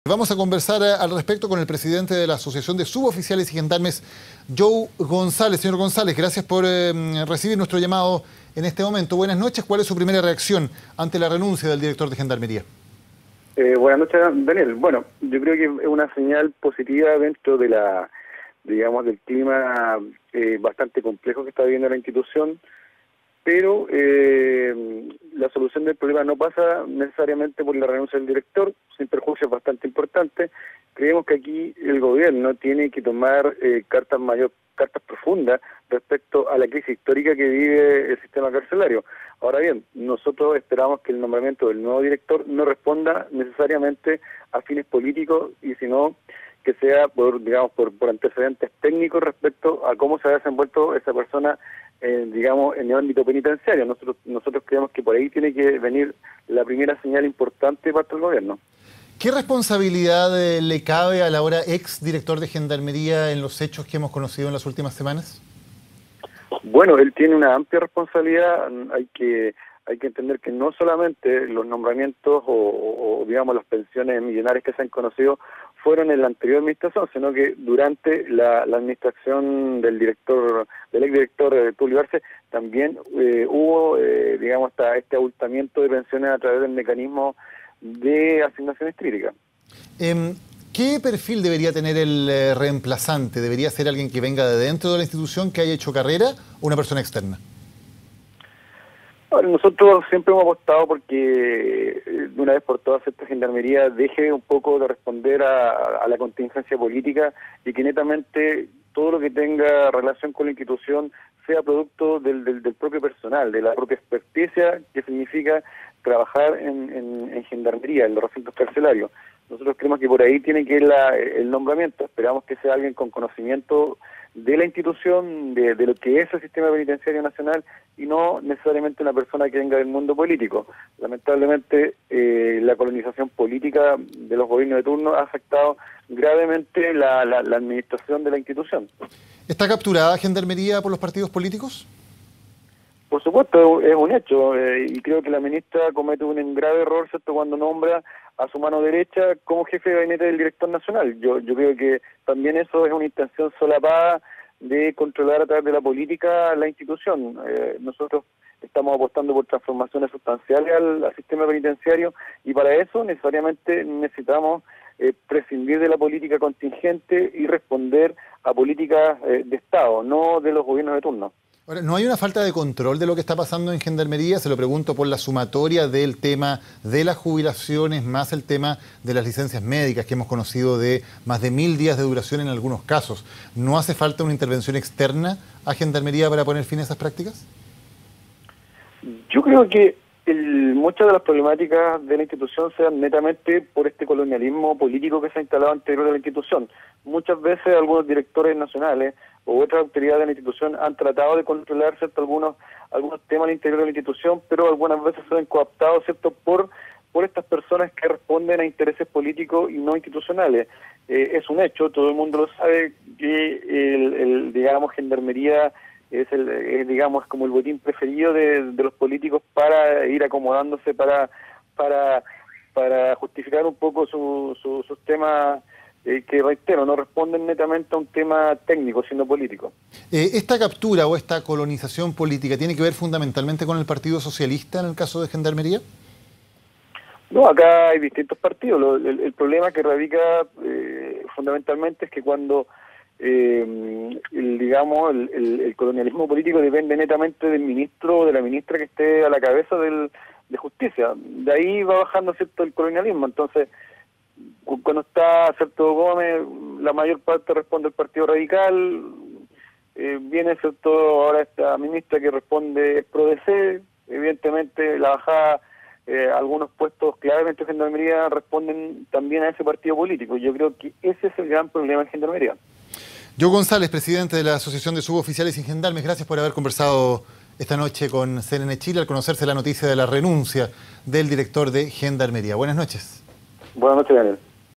Joe González y renuncia del director de Gendarmería: "Es una señal positiva"
Además sostuvo que el nuevo director "no necesariamente debe venir del mundo político".